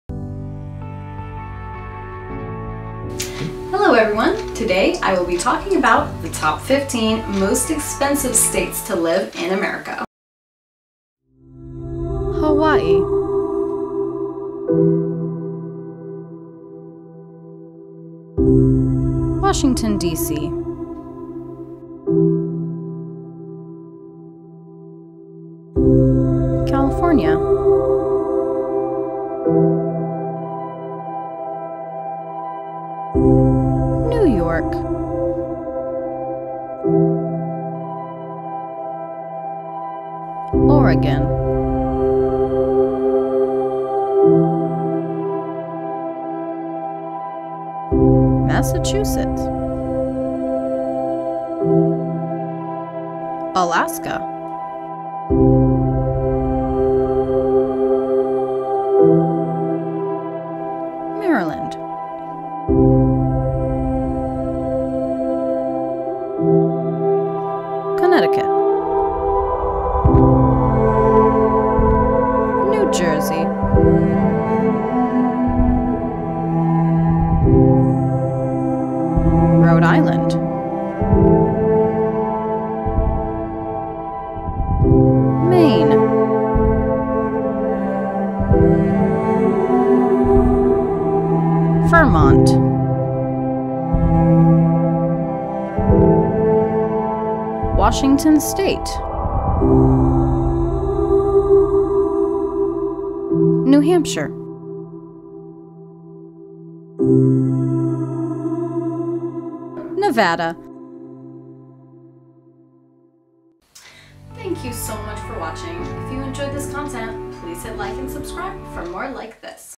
The Top 15 Most Expensive States To Live In America 2022!
Do you want to live in any of these places??

This is a short compilation of the 15 most expensive states to live in America as of 2022! I included living wage, median income, and median house price. It's an interesting set of data. Let me know what you think!

 My sources and social media are linked below.

0:00 intro
0:12 Hawai'i
0:19 Washington D.C.
0:26 California
0:34 New York
0:40 Oregon
0:48 Massachusetts
0:53 Alaska
1:01 Maryland
1:07 Connecticut
1:13 New Jersey
1:19 Rhode Island
1:25 Maine
1:32 Vermont
1:38 Washington
1:43 New Hampshire
1:49 Nevada
1:53 outro